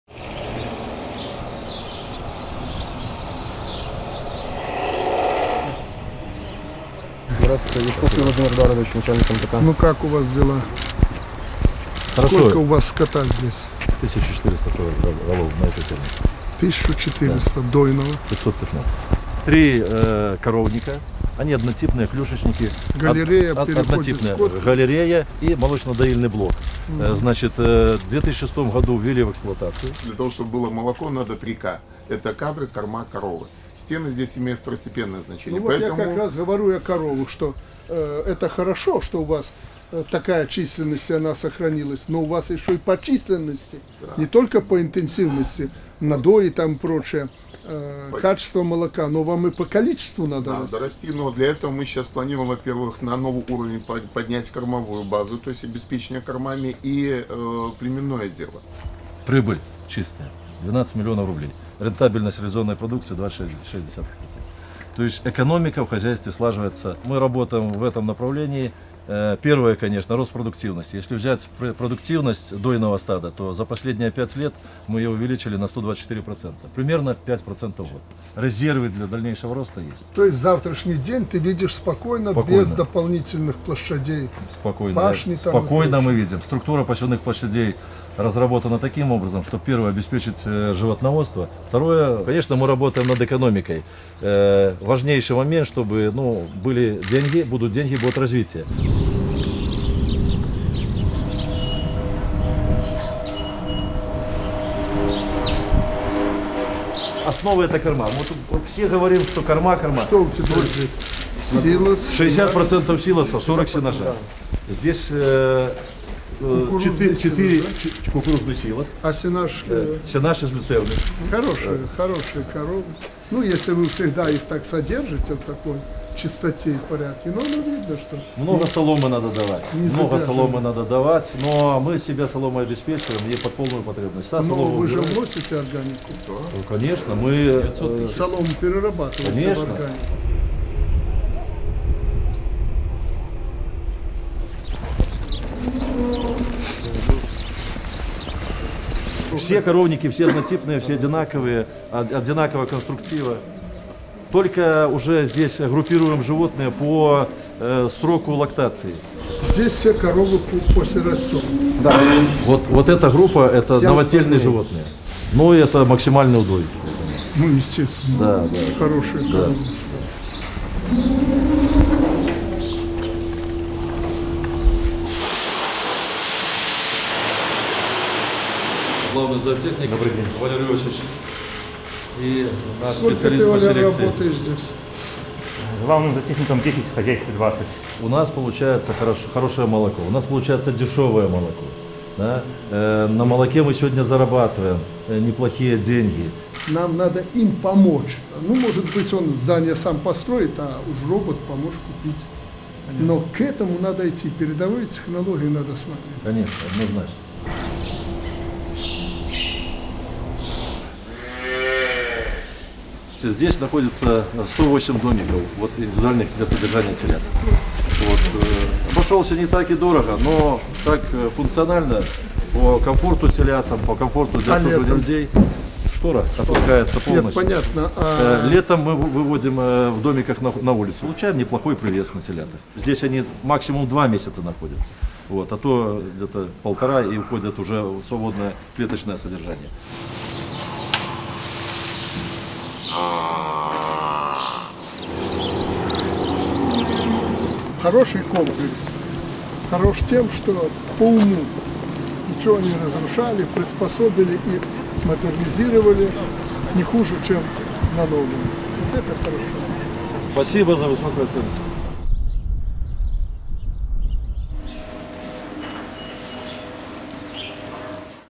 Лукашенко посетил сельхозпредприятие под Гродно с минским уровнем зарплат
Президент Беларуси Александр Лукашенко во время рабочей поездки в Гродненскую область посетил молочно-товарный комплекс "Обухово" СПК имени И.П.Сенько.